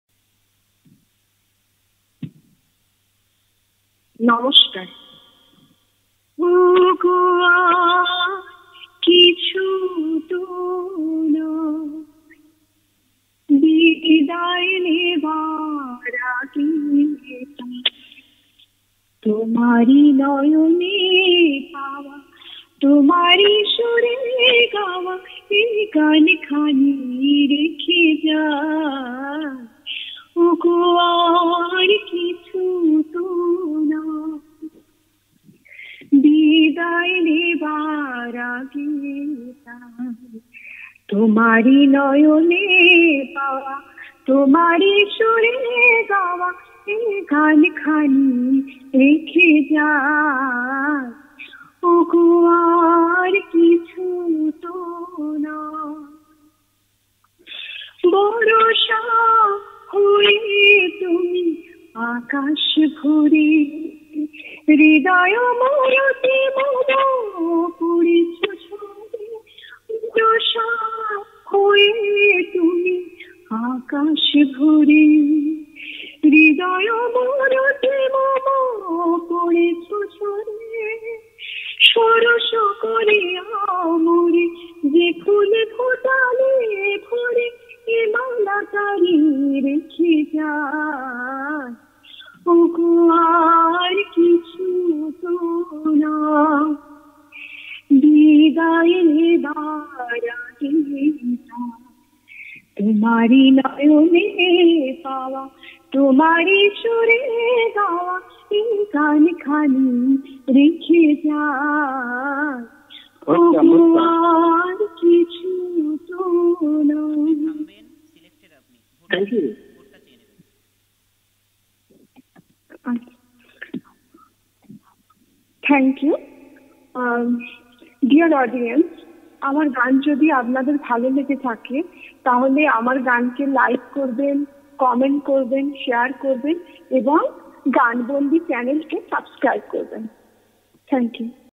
Mousumi Das - Modern Songs Competition (6th - 9th June 2020)
Organised by GAAN-BONDEE (NW) Musical Adda. Please support our candidates as they can sing the songs with more inspiration.